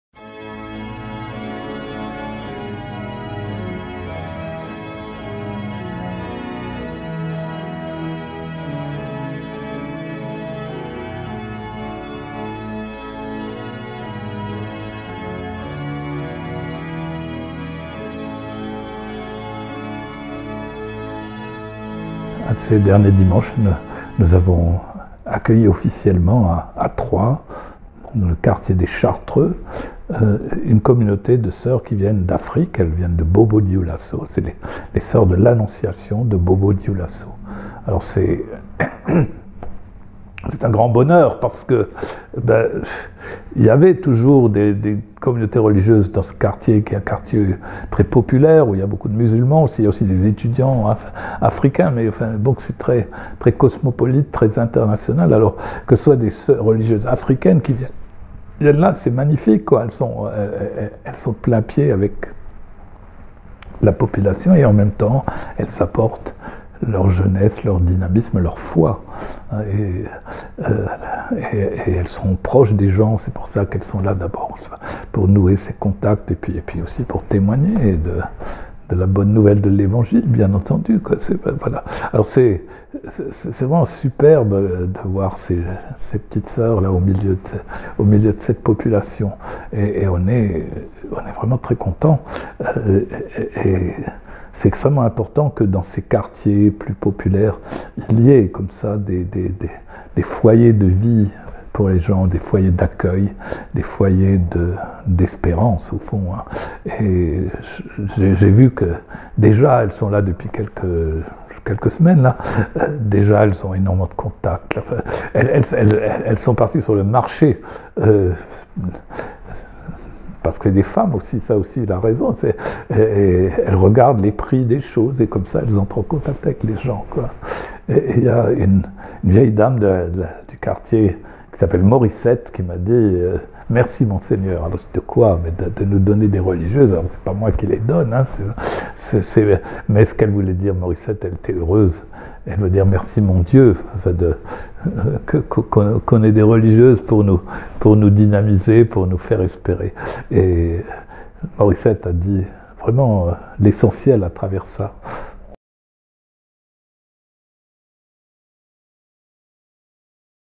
Retour sur l'arrivée des 3 Soeurs de l'Annonciation de Bobo-Dioulasso
Mgr Marc Stenger exprime sa joie d'accueillir une nouvelle communauté religieuse, au milieu de la population troyenne